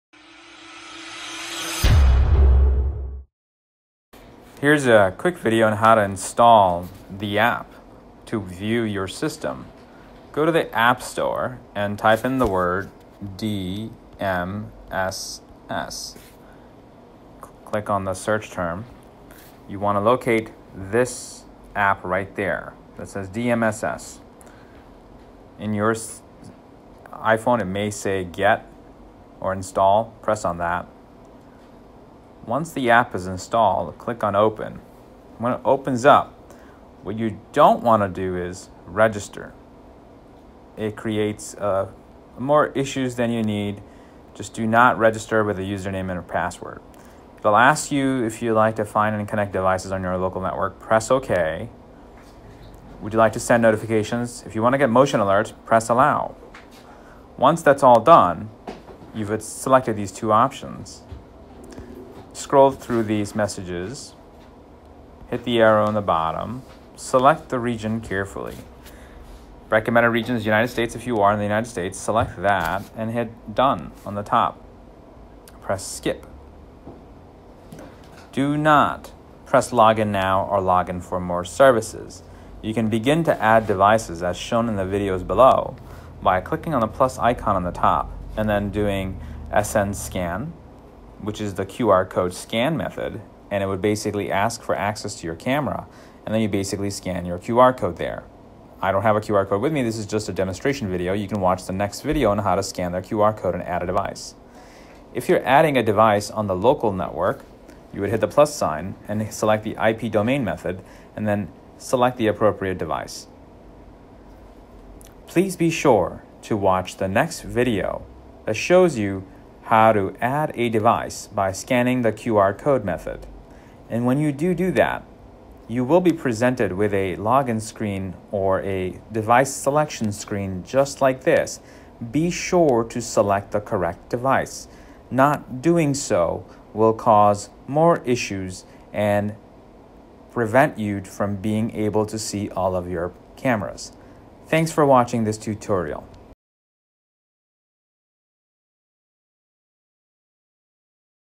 How to Install the Remote Viewing App
In this video our tech shows you how to install the remote viewing app. Creating an account in the app binds the system to the email account, and for security purposes the app does not allow more than one user to access the email account at the same time. We do not recommend creating an account in the app as it can prevent viewing from multiple devices at the same time.